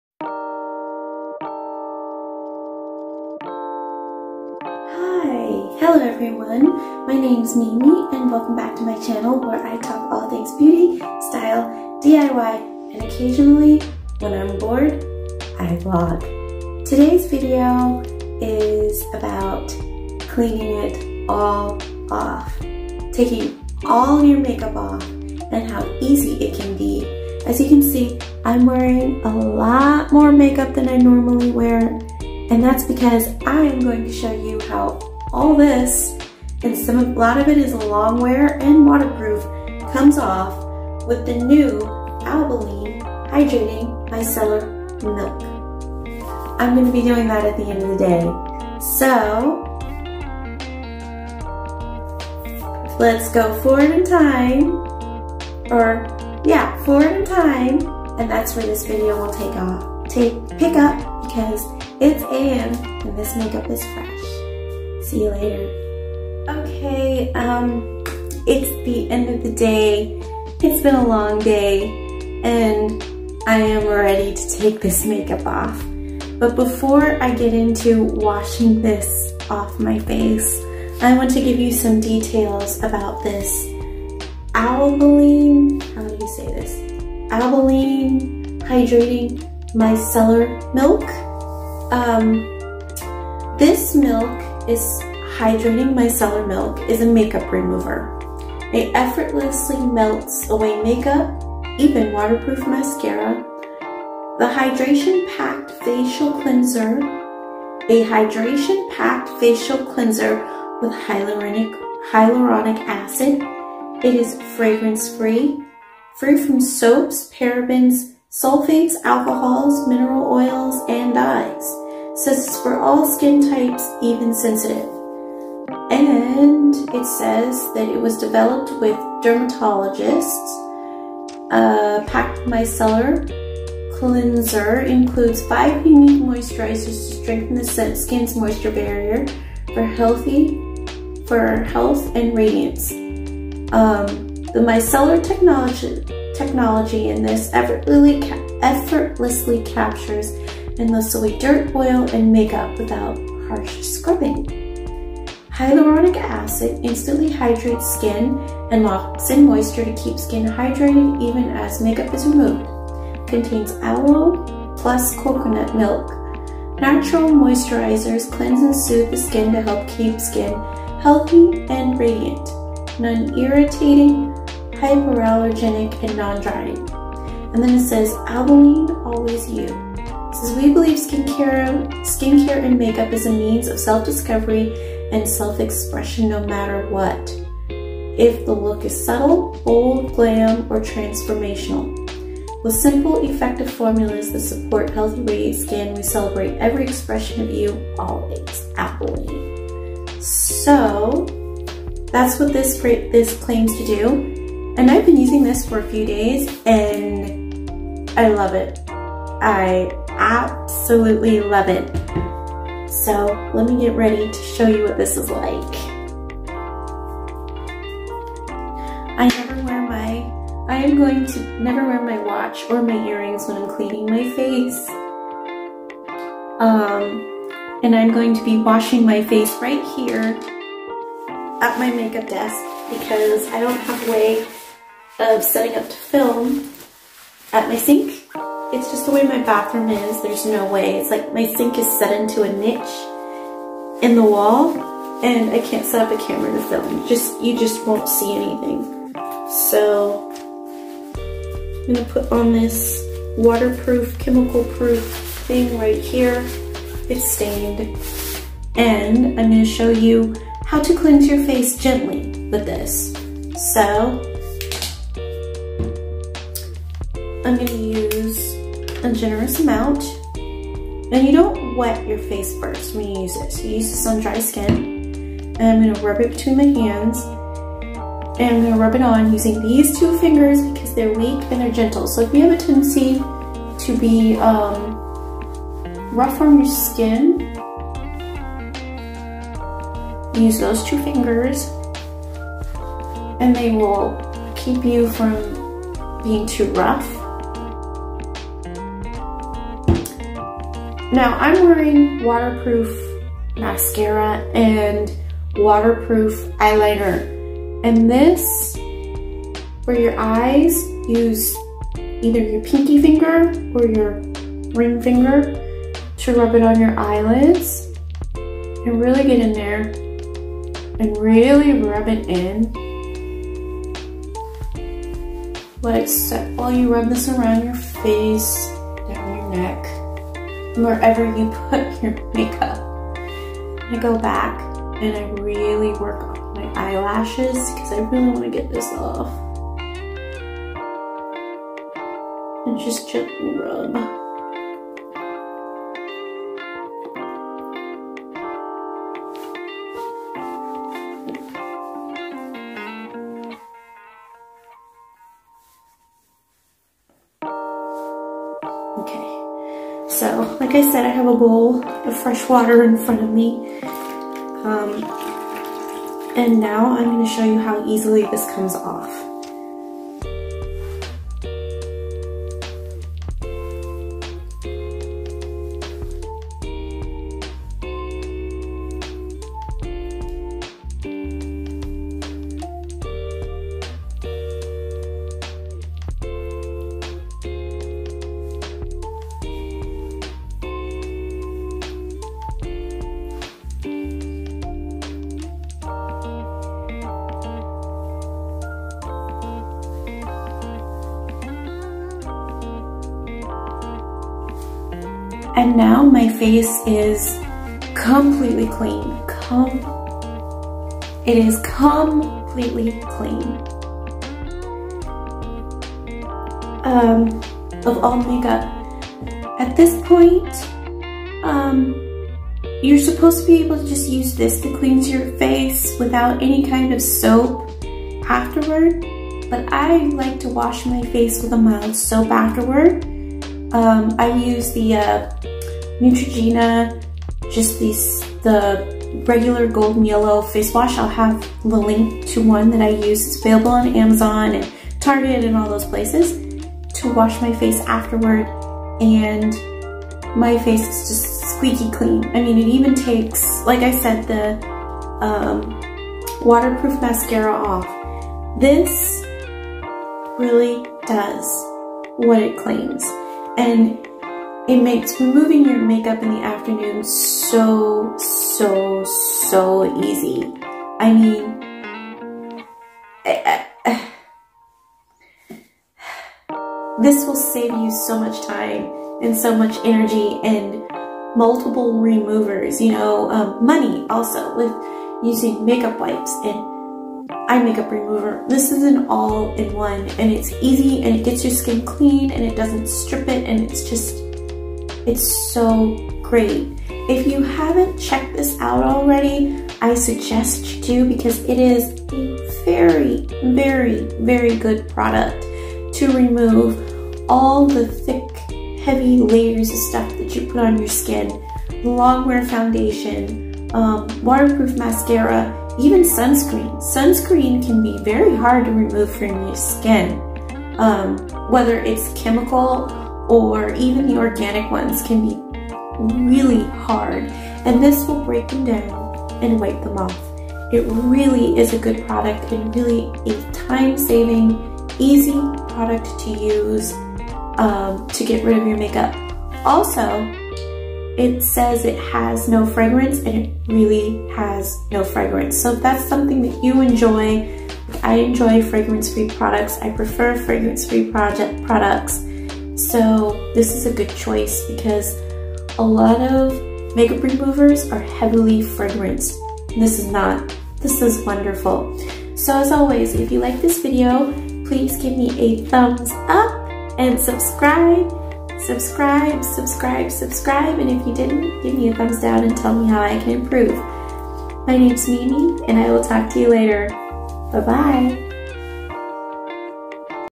Albolene Miceller Makeup Milk Review
A review of the Miceller Makeup Milk from Albolene.